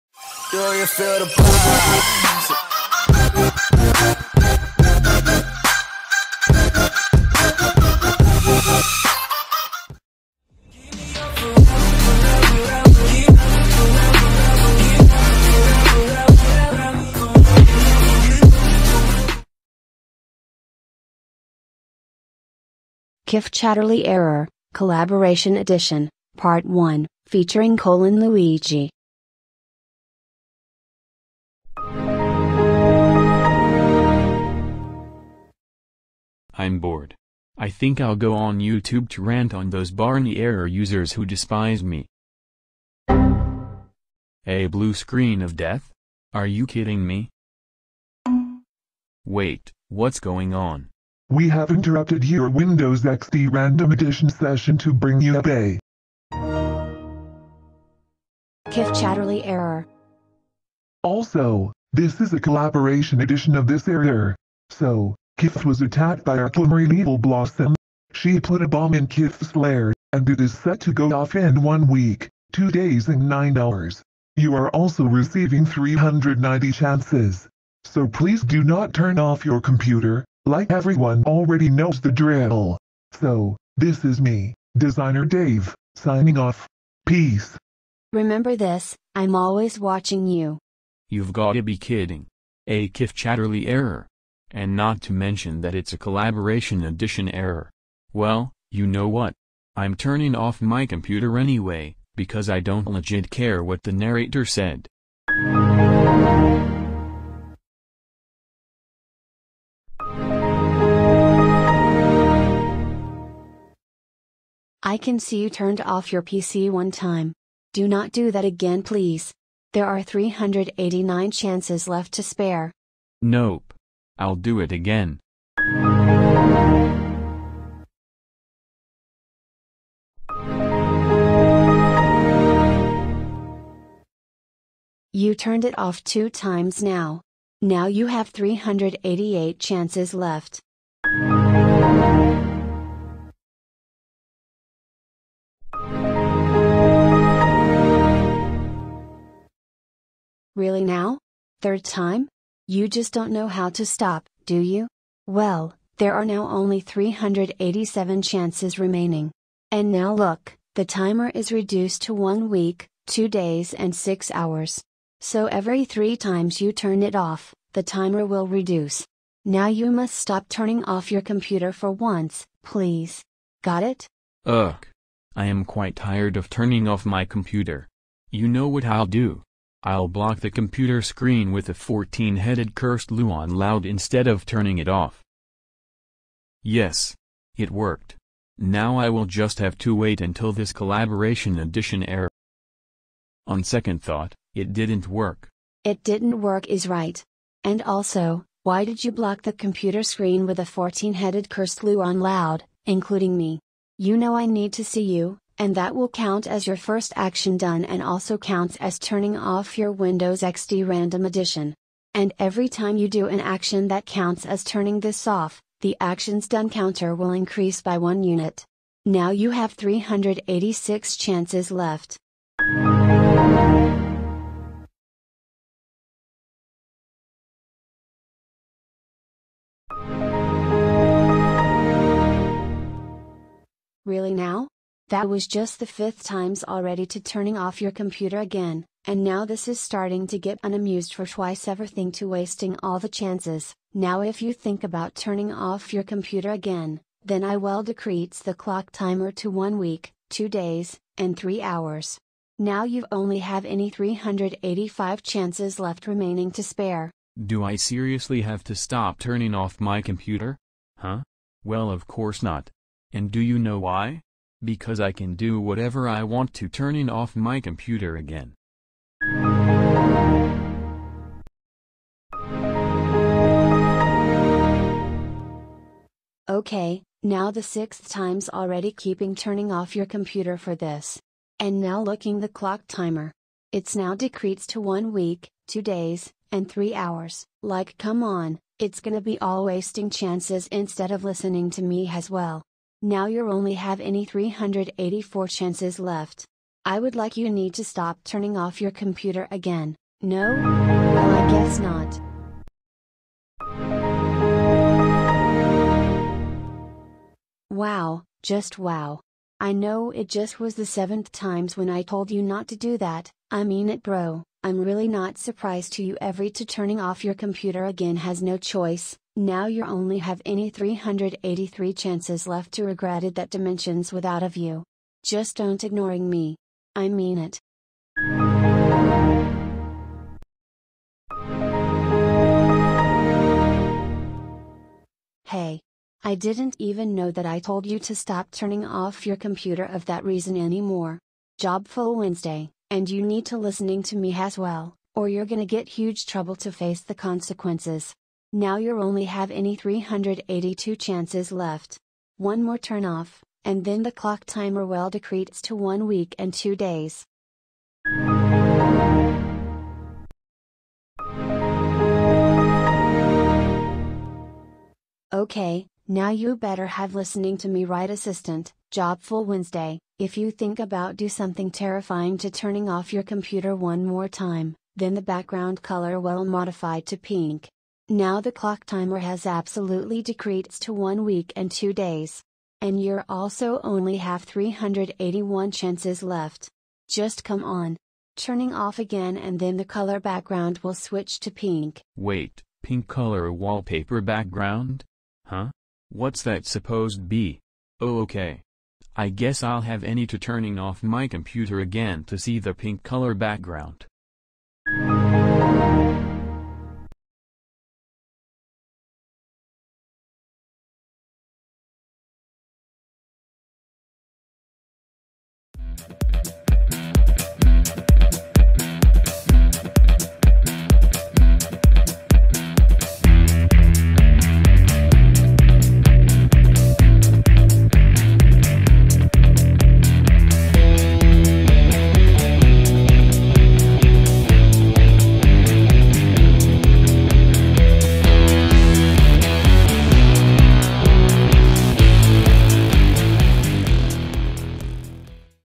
Kiff Chatterley Error (Collaboration Edition) [Part 1] {Ft. Cole and Luigi}
Part 1 Is Finished To The Point Where JobfulWednesday Just Got An Error As A Collaboration Edition Version of It With Kiff's Normal Mode, The White Background and The Segoe UI Fonts. And Special Thanks To @6890chris and @LuigharyTheVideoGamesBECM2025 For Making This For Me. @6890chris and @DEaSC2025Reborn, Your Turn To Make Part 2 (Which Cole Will Make The First Half of Part 2 (Chances 380-376) and Reborn Will Make The Second Half of Part 2 (Chances 375-371) Afterwards), Because That's Coming Up Next.